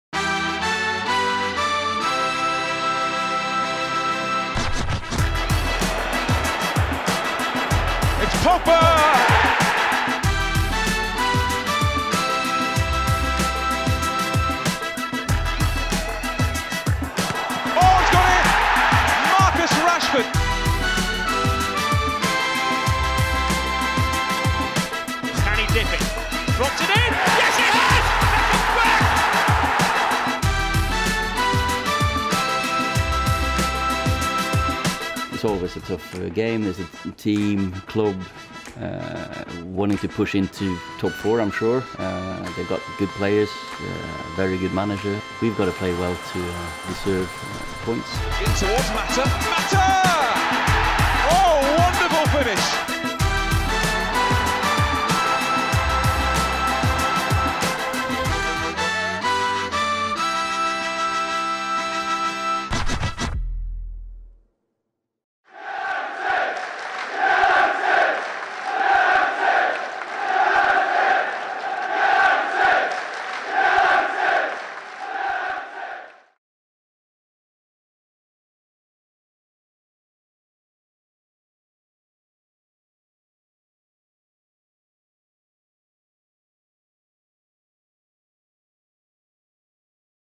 Preview | Manchester United v Leicester City | Premier League
Manchester United take on Leicester City in the Premier League this afternoon, with Ole Gunnar Solskjaer's side looking to get back to winning ways after the international break.